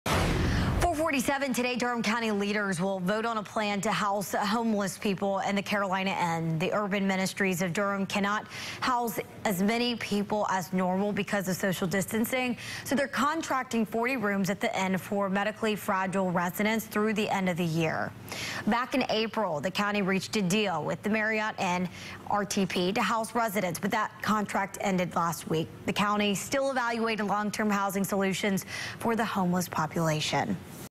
Durham County leaders to vote on contract with Carolina Inn to house homeless
Urban Ministries of Durham cannot house all the homeless people due to social distancing restraints so they have been working to find hotels to house the homeless.